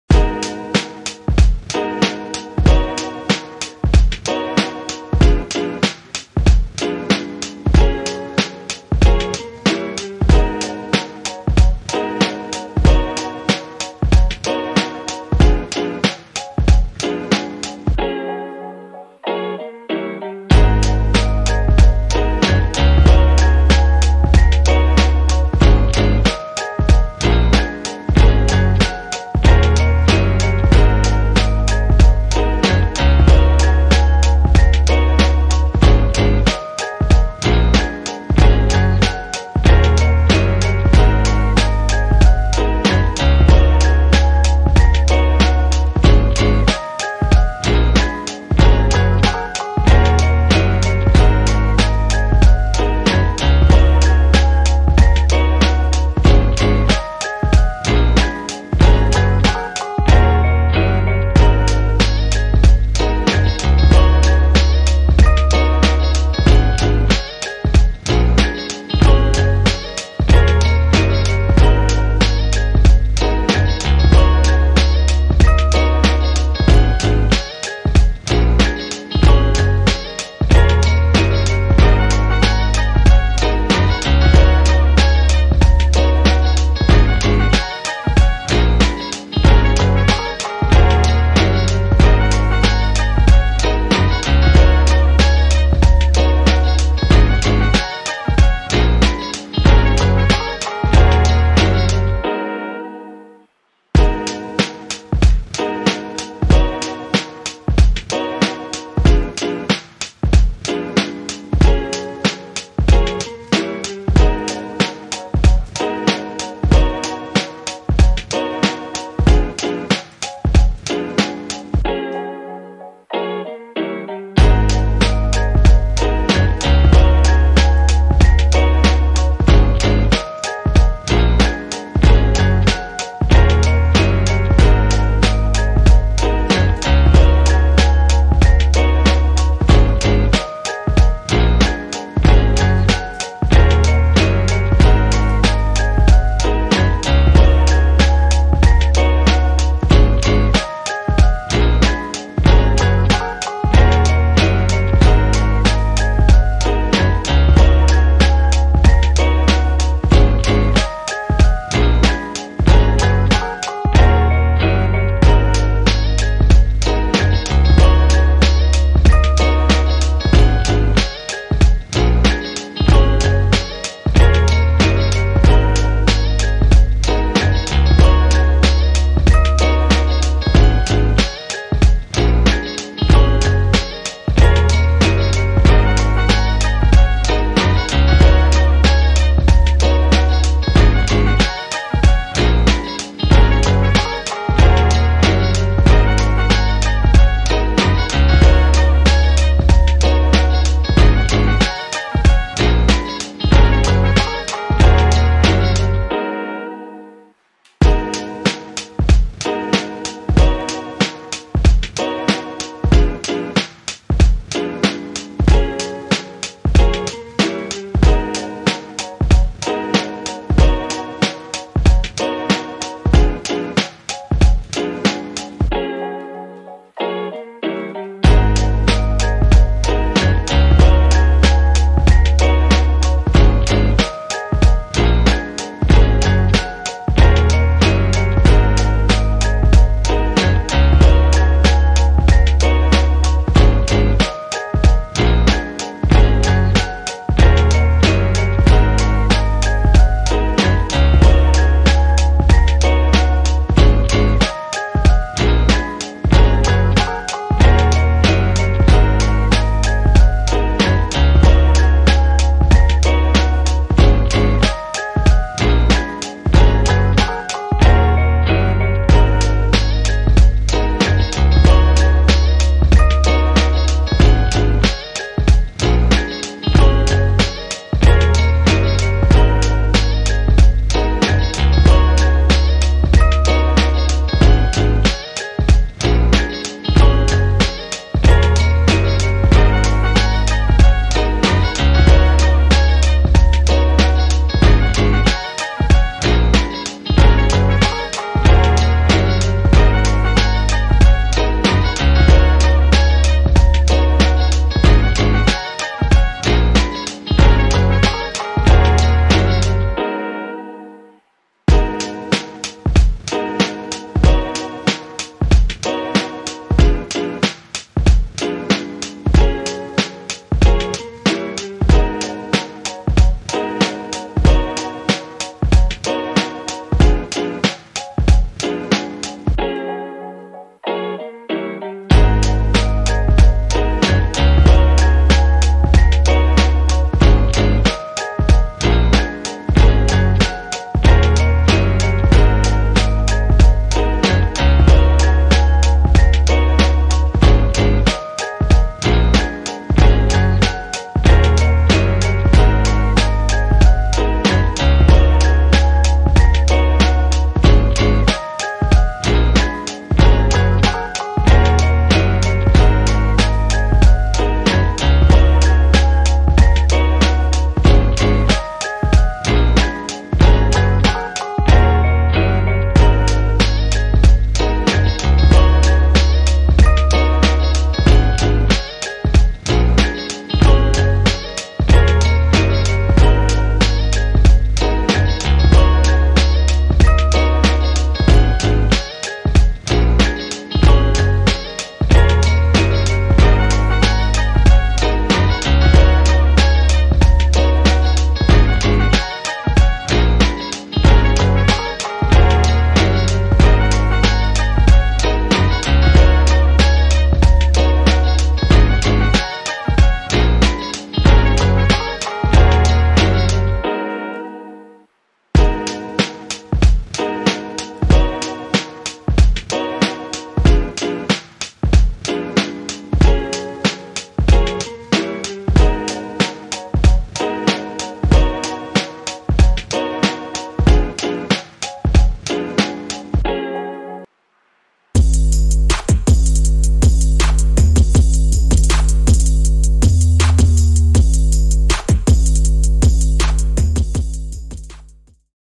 Solving all of my puzzles
I solved all my available cubes in less than 6 minutes.